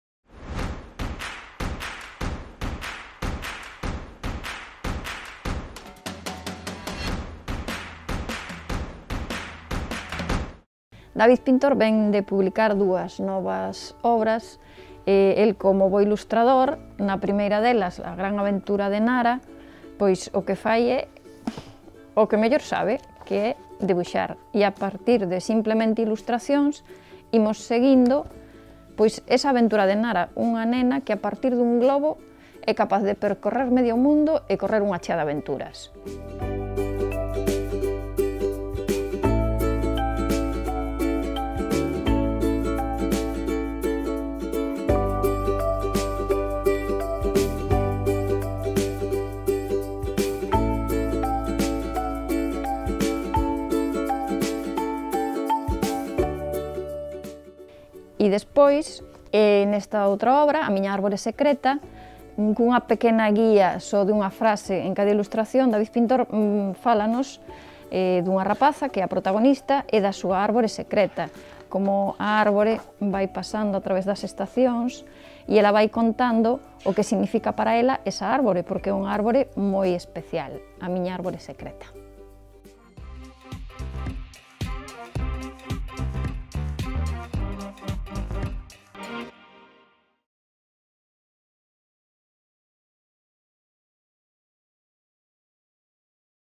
Libraría Couceiro: 'La gran aventura de Nara' e 'A miña árbore secreta'
A Libraría Couceiro recomenda hoxe dúas publicacións do ilustrador galego David Pintor. Por un lado, 'La gran aventura de Nara' que, só mediante debuxos nos conta a gran aventura desta rapaza e o seu globo vermello.
Polo outro, María recoméndanos 'A miña árbore secreta', un conto que nos fala dunha rapaza e a súa árbore secreta que ao longo da súa vida vai significando cousas diferentes para ela.